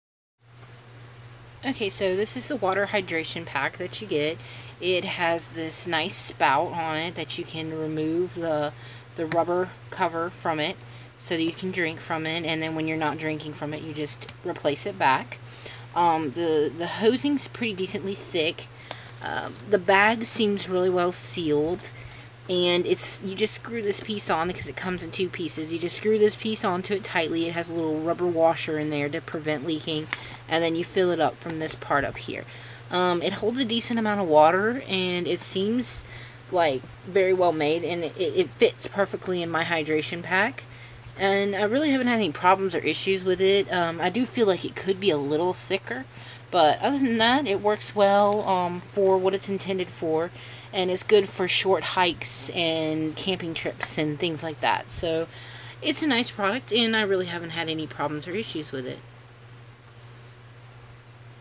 Hydration Bladder Water Reservoir Pack + FREE Survival Keychain Whistle, hydrant 2L Bag is BPA Free,
This Hydration Bladder Water Reservoir Pack is decently durable and seems to be well made of nice materials. It is a nice size and fits in almost any hydration backpack so it's very universal for sure. All the valves are very well attached and do not leak at all even under the pressure of the water even when filled up completely. You can easily remove parts to clean and sanitize all the parts and hydration pack for multiple uses. It's also very light weight so it doesn't add a ton of weight which is great because when hiking you want to limit the amount of weight you have to carry. I like the amount of water you get when using it too every time you use the valve it provides a decent amount of water. Im happy with this product and the way it works. I will note this did NOT come with the whistle as advertised in the listing I don't know how or why but it was not with it at all. It is exactly what I was expecting and is accurately described in the description. I have not had any issues with this product it has preformed perfectly. I would recommend this to any person who loves to hike or that just wants to carry a hydration pack that will meet all their needs.